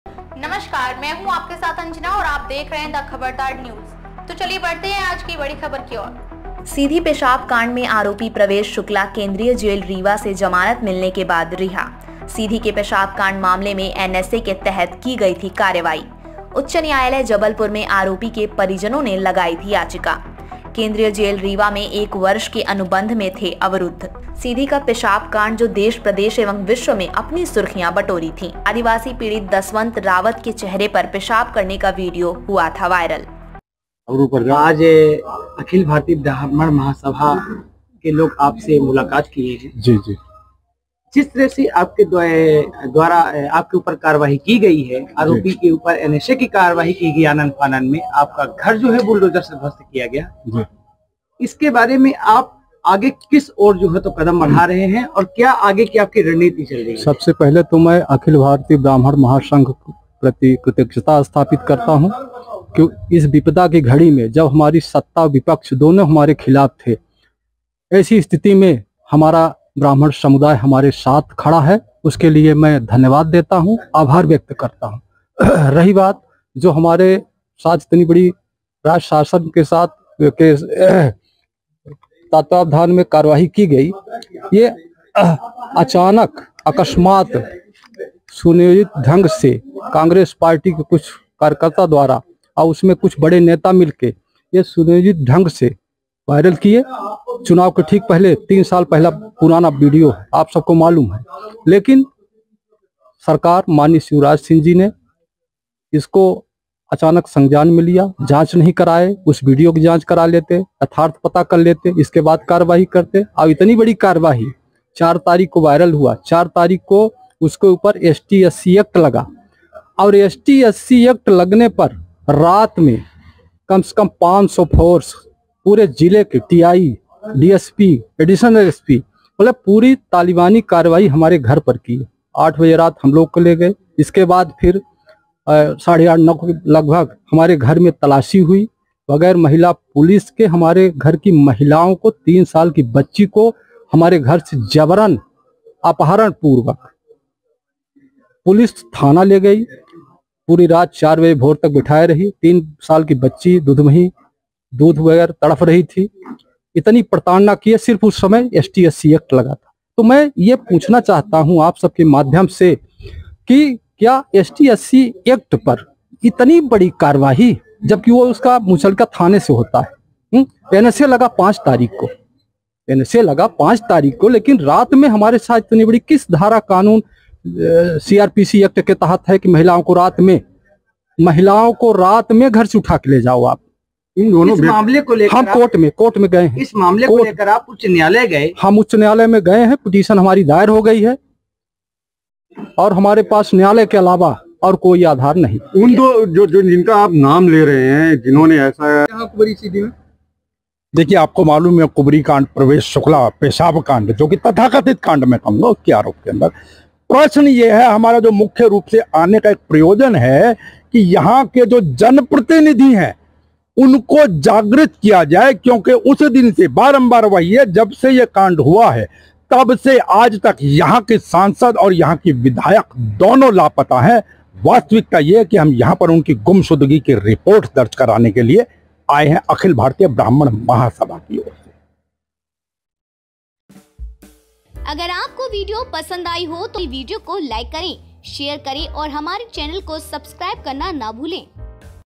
Sidhi के पेशाब कांड का आरोपी Pravesh Shukla, Central Jail से रिहा। NSA। MP Election 2023.
Sidhi News: Sidhi के पेशाब कांड का आरोपी  Pravesh Shukla, Central Jail से रिहा। NSA। MP Election 2023.
सीधी पेशाब कांड में आरोपी प्रवेश शुक्ला केंद्रीय जेल रीवा से जमानत मिलने के बाद रिहा
सीधी के पेशाब कांड मामले में एन एस ए के तहत की गई थी कार्रवाई
उच्च न्यायालय जबलपुर में आरोपी के परिजनों ने लगाई थी याचिका
केंद्रीय जेल रीवा में 1 वर्ष के अनुबंध में थे अवरुद्ध
सीधी का पेशाब कांड जो देश प्रदेश एवं विश्व में अपनी सुर्खियां बटोरीं थी
आदिवासी पीड़ित दसवंत रावत के चेहरे पर पेशाब करने का वीडियो हुआ था वायरल.